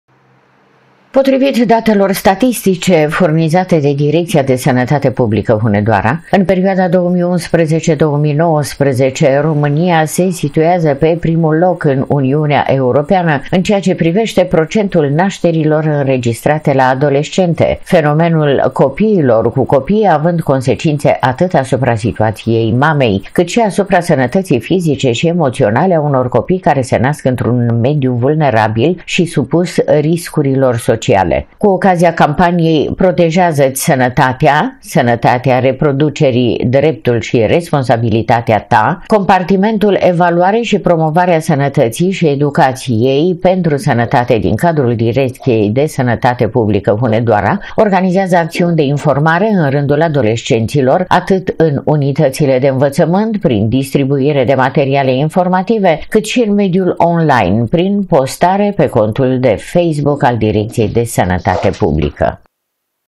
ROMÂNIA, PE PRIMUL LOC ÎN U.E. LA FENOMENUL ”COPII CU COPII”
Potrivit datelor statistice, furnizate de Direcția de Sănătate Publică Hunedoara, în perioada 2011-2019, România se situează pe primul loc în U.E. în ceea ce privește procentul nașterilor înregistrate la adolescente, fenomenul „copiilor cu copii” având consecințe atât asupra situației mamei, cât și asupra sănătății fizice și emoționale a unor copii care se nasc într-un mediu vulnerabil și supus riscurilor sociale. Cu ocazia campaniei „Protejează-ți sănătatea!  Sănătatea reproducerii – dreptul și responsabilitatea ta!”, Compartimentul Evaluare și Promovare a Sănătăţii şi Educaţie pentru Sănătate din cadrul Direcţiei de Sănătate Publică Hunedoara, organizează acțiuni de informare în rândul adolescenților, atât în unitățile de învățământ, prin  distribuire de materiale informative, cât și în mediul online, prin postare pe contul de facebook al Direcției de Sănătate Publică.